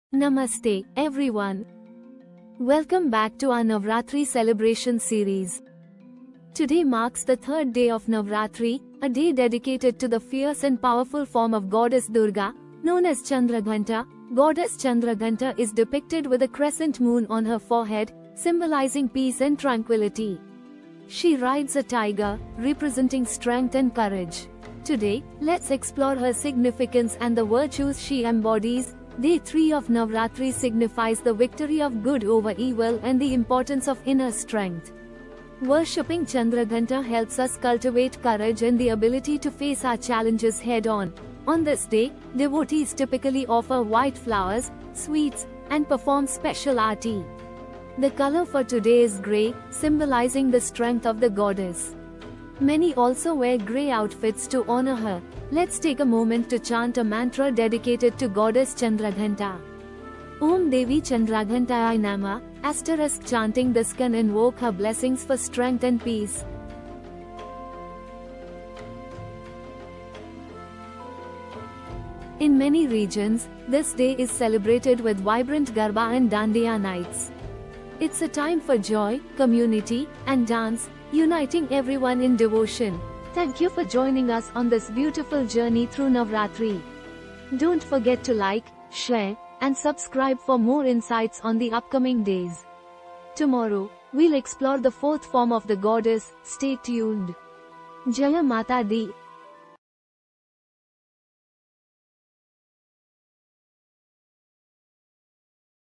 Viral Youtube Title: Navratri 2024: 3rd Day Celebrations Explained in Detailed#motivation navrati.
Today, we dive into the vibrant celebrations of the third day of Navratri. This day is dedicated to the goddess Chandraghanta, symbolizing bravery and strength.
On this auspicious day, devotees worship her by performing traditional rituals, including floral offerings and chanting powerful mantras. The sound of bells and the fragrance of incense fill the air, creating a divine atmosphere.
People dress in shades of yellow, representing happiness and prosperity. The dances, especially Garba and Dandiya, bring communities together, as everyone joins in the rhythmic celebration.
Food plays a vital role too. Special dishes prepared include dishes like Sabudana Khichdi and Sabudana Vada, which are enjoyed by many during fasting.
As we celebrate this day, remember to embrace the spirit of devotion, unity, and joy that Navratri brings.